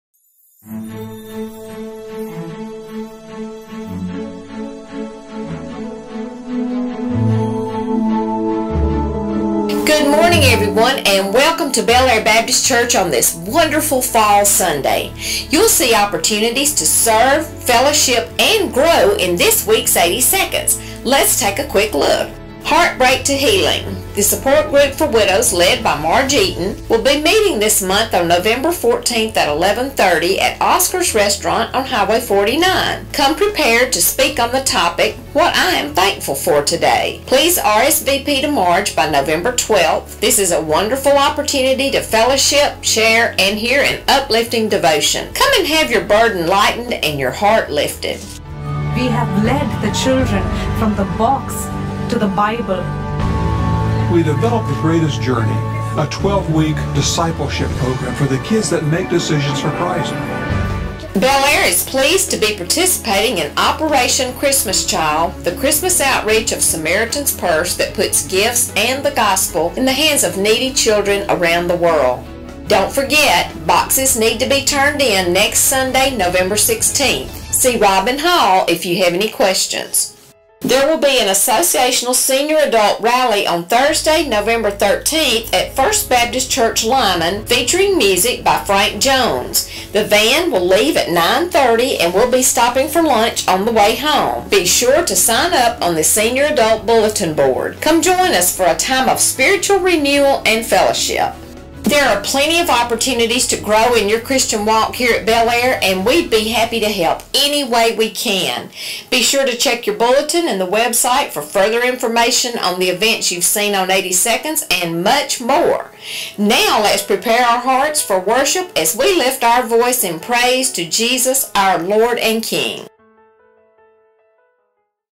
Episode #271 Around Bel-Aire in 80 seconds Nov 9
Heartbreak to Healing, Operation Christmas Child, Sr. Adult Rally at FBC Lyman